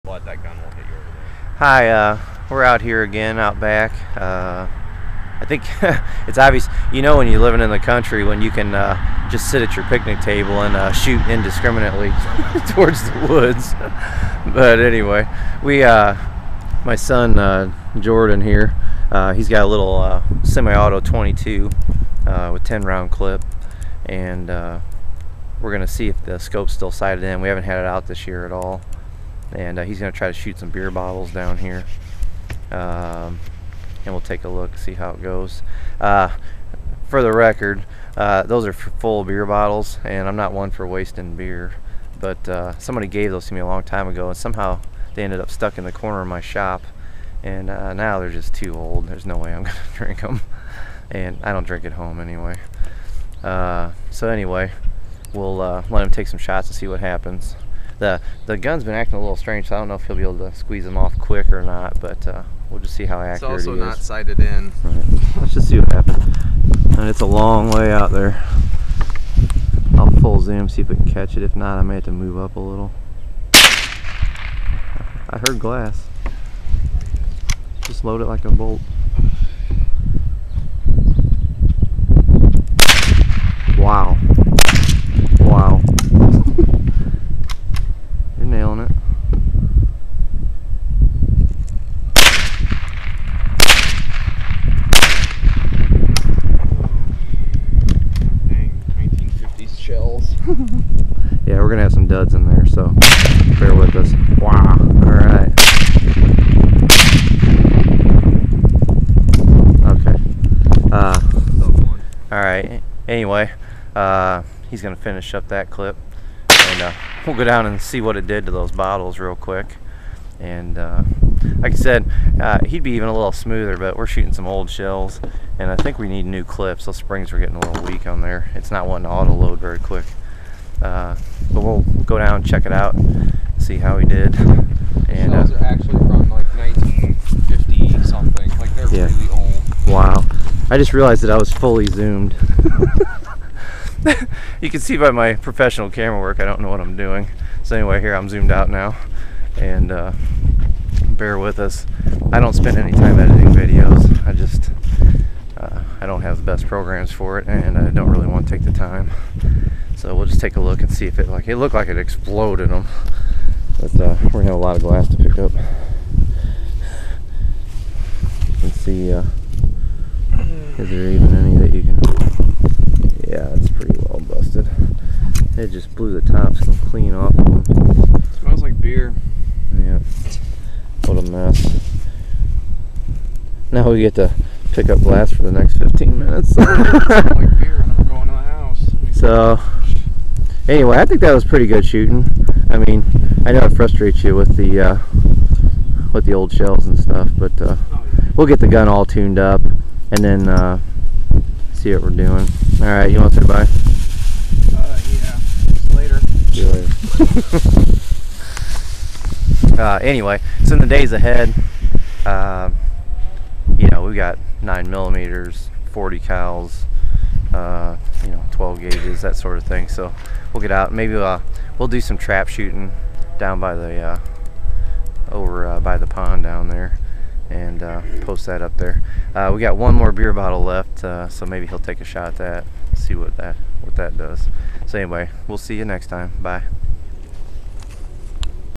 Jordan Shooting and Exploding Some Bottles with his .22 Semi-Auto Rifle
Jordan got his rifle out for the first time this year. We were having some issues like the scope being slightly off, old ammo, weak springs in the clips, but it was still fun. Atleast you get to see some beer bottles explode. We'll get the gun all tuned up, get some new ammo, and do some better vids in the future.  He really is a deadly shot with everything properly sorted out.